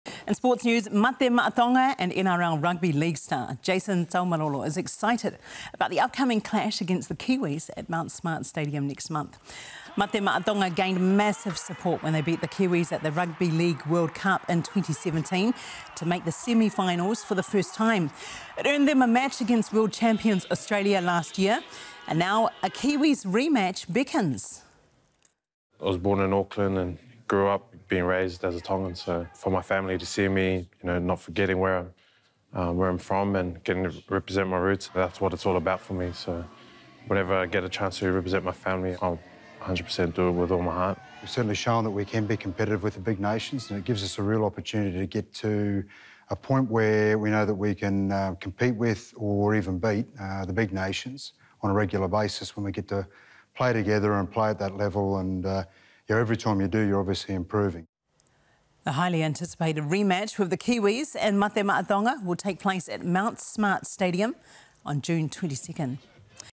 Jason Taumalolo excited about Mate Ma’a Tonga clash against Kiwis
“For my family to see me not forgetting where I’m from and getting to
represent my roots, that’s what it’s all about for me.”